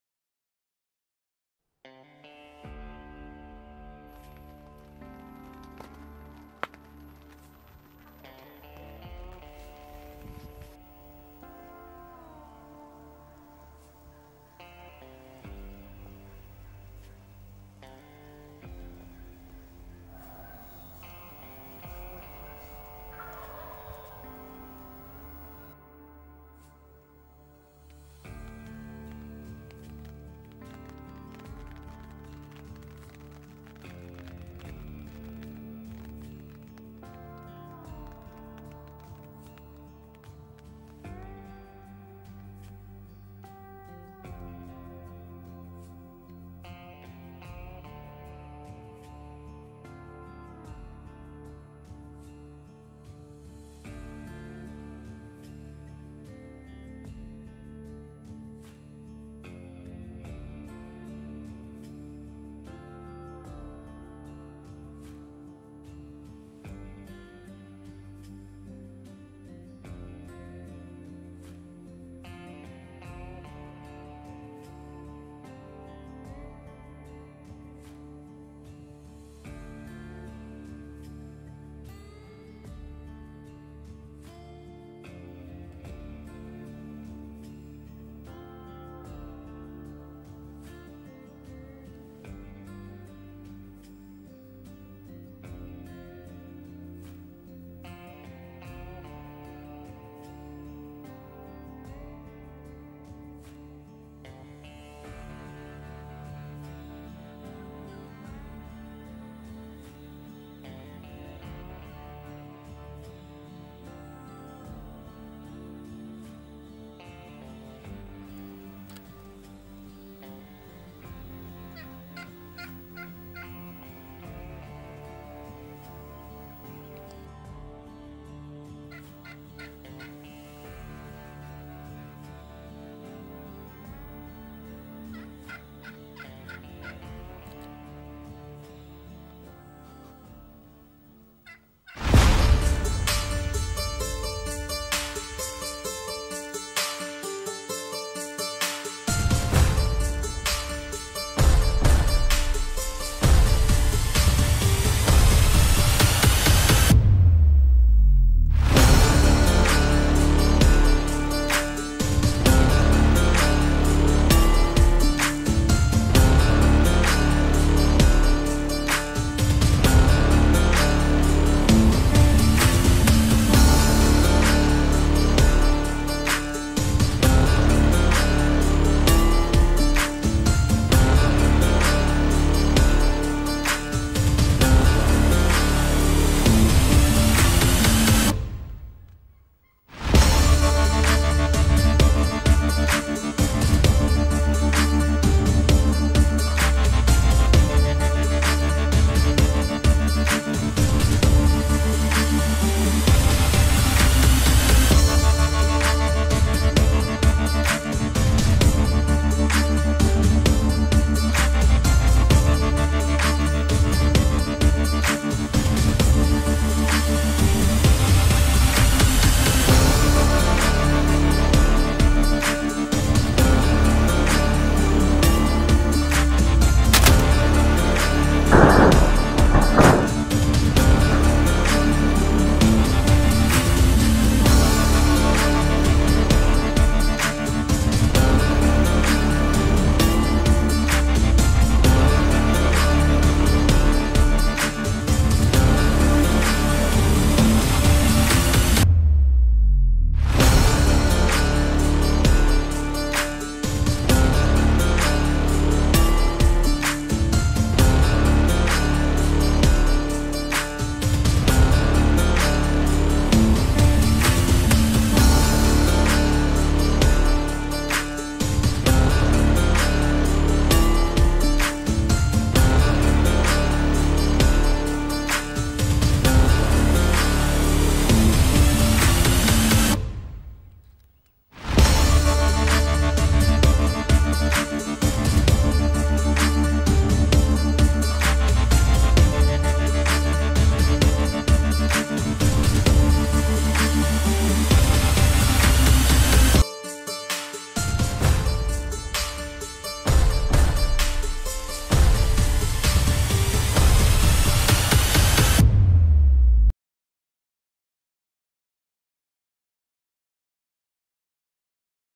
Post rain gobbler! (Ontario turkey hunting)
Opening weekend proved to be successful in the rain!